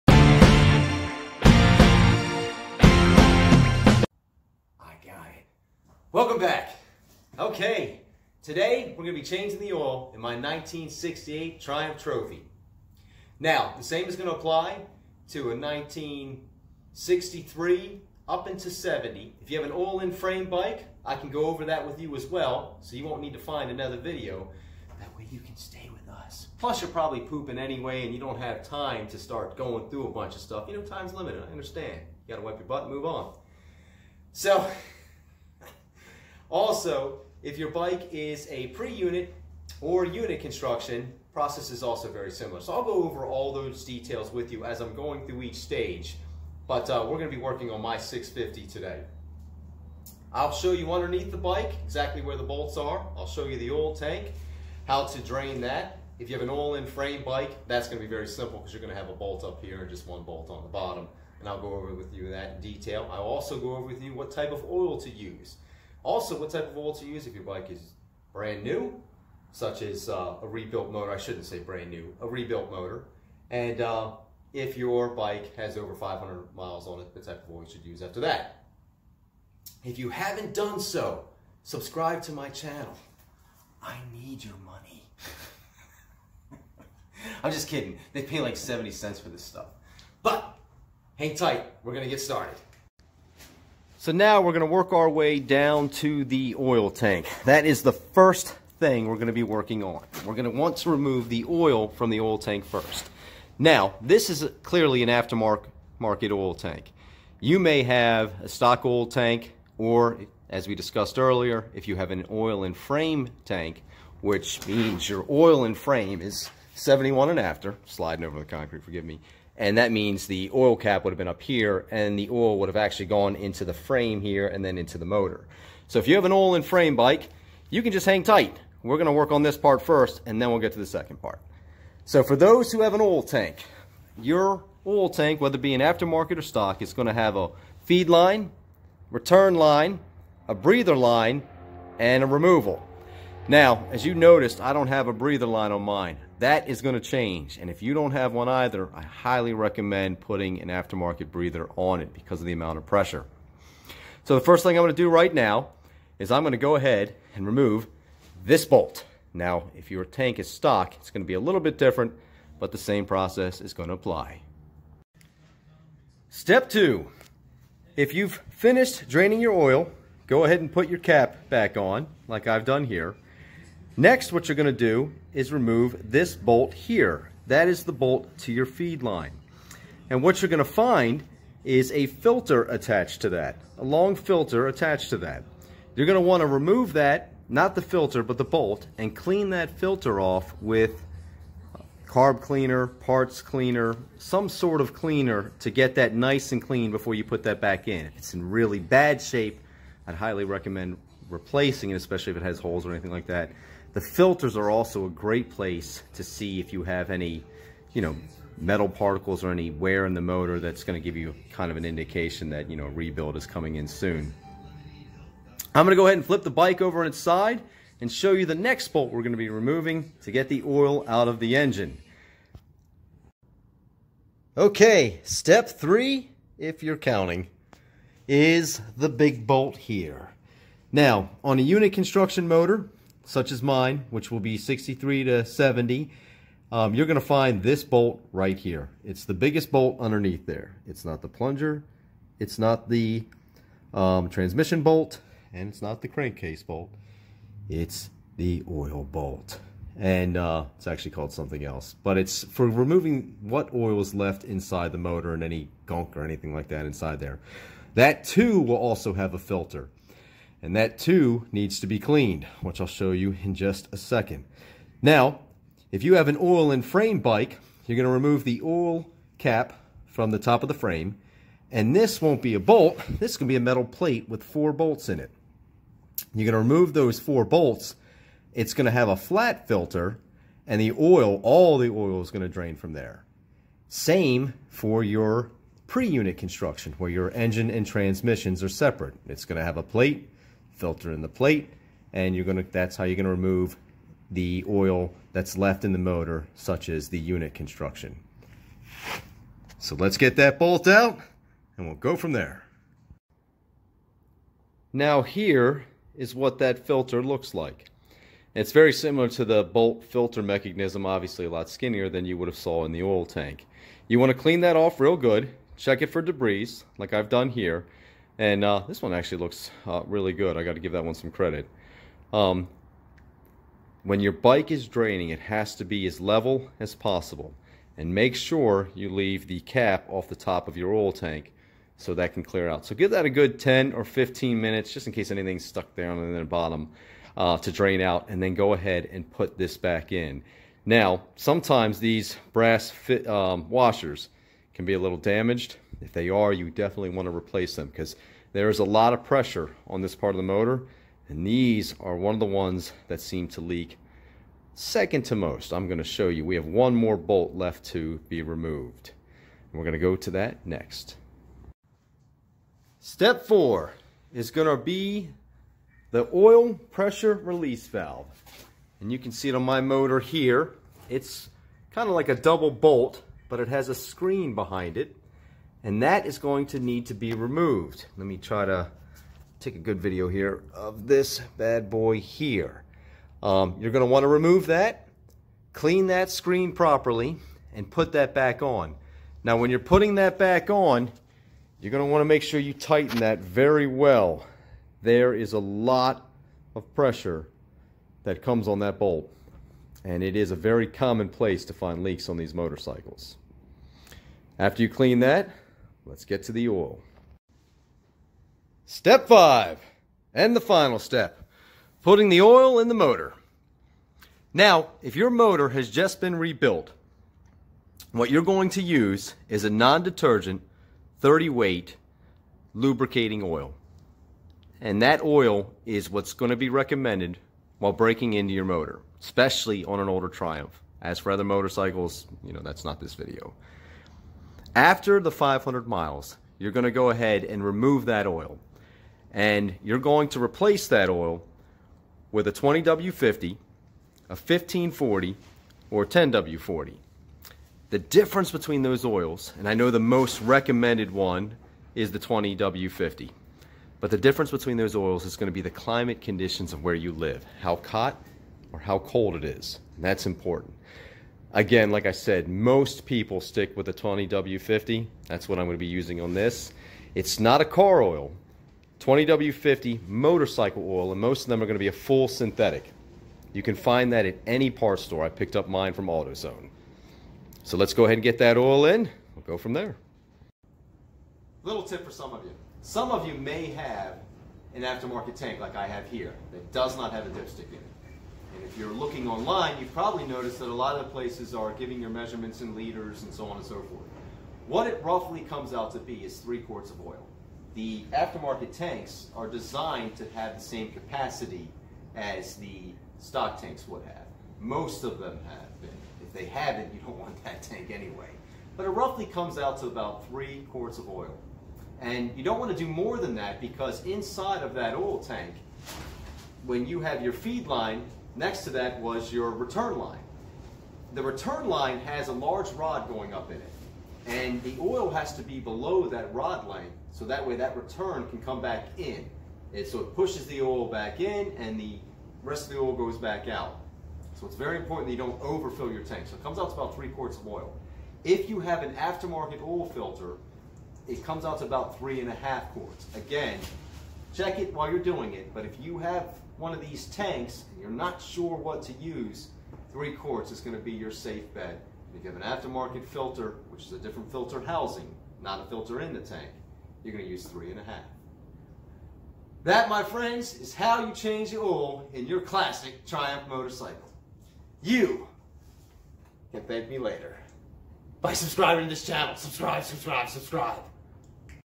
Oil change in a classic Triumph Motorcycle. 500,650 and 750
oil change in my 1968 triumph Tr6 motorcycle. We will go over the type of oil. The amount of oil. All filters that need to be removed and cleaned. video of all the bolt locations to mae this as easy as possible. check out my other videos for the gearbox and chain case.